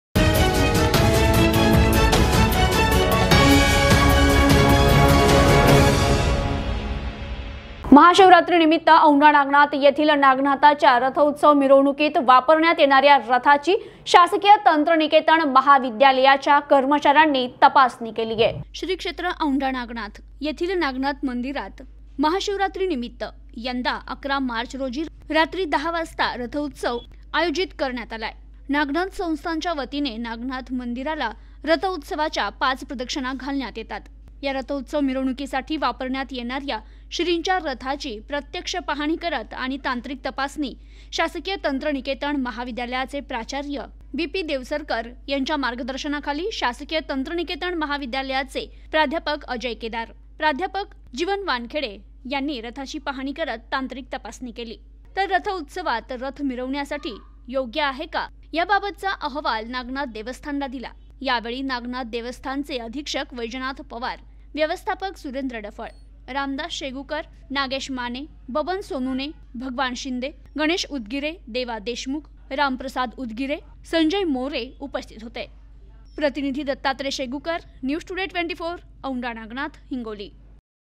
औंढा नागनाथ : महाशिवरात्रीनिमित्त नागनाथ मंदिरात शासकीय तंत्रनिकेतन महाविद्यालयाकडून रथाची तपासणी
औंढा नागनाथ : महाशिवरात्रीनिमित्त नागनाथ मंदिरात रथोत्सव मिरवणुकीसाठी वापरण्यात येणाऱ्या श्रीच्या रथाची शासकीय तंत्रनिकेतन महाविद्यालयाच्या  कर्मचाऱ्यांनी तपासणी केली

शासकीय तंत्रनिकेतन महाविद्यालयाकडून रथाची तपासणी